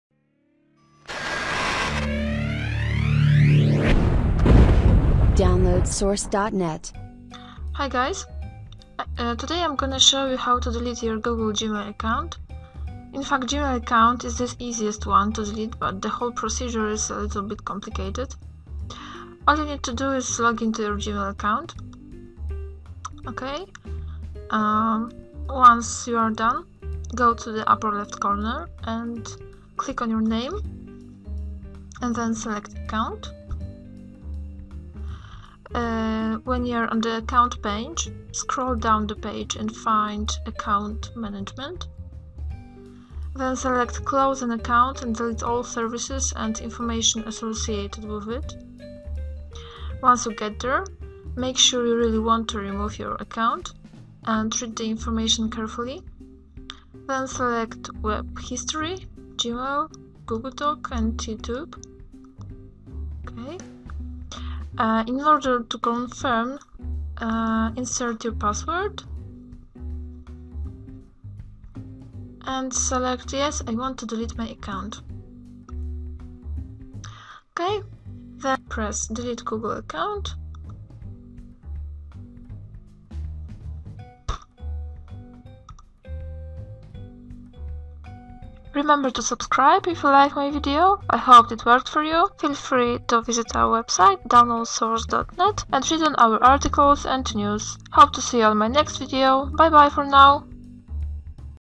How to delete gmail account permanently? (2013)
In this video I'd like to show you, how to delete gmail account permanently. Before start you must be sure that you really want to remove yout Google account.